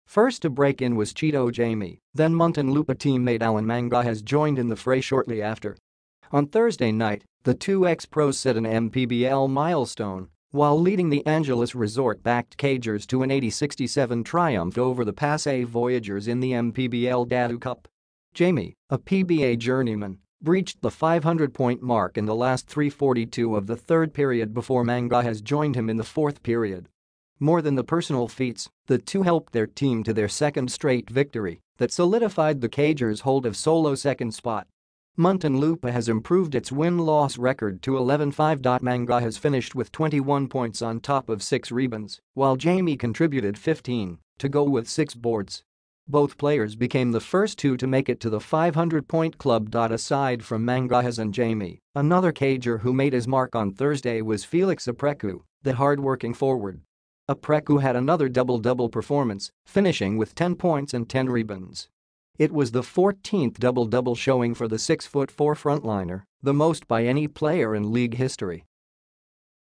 500-Point Club | Jaime, Mangahas first to achieve MPBL milestone in ‘Munti’s win
First to break in was Chito Jaime, then Muntinlupa teammate Allan Mangahas joined in the fray shortly after.

On Thursday night, the two ex-pros set an MPBL milestone while leading the Angelis Resort-backed Cagers to an 80-67 triumph over the Pasay Voyagers in the MPBL Datu Cup.

Jaime, a PBA journeyman, breached the 500-point mark in the last 3:42 of the third period before Mangahas joined him in the fourth period.

More than the personal feats, the two helped their team to their second straight victory that solidified the Cagers’ hold of solo second spot. Muntinlupa has improved its win-loss record to 11-5.

Mangahas finished with 21 points on top of six rebounds while Jaime contributed 15 to go with six boards.

Both players became the first two to make it to the 500-point club.

Aside from Mangahas and Jaime, another Cager who made his mark on Thursday was Felix Apreku, the hardworking forward.

Apreku had another double-double performance, finishing with 10 points and 10 rebounds. It was the 14th double-double showing for  the 6-foot-4 frontliner, the most by any player in league history.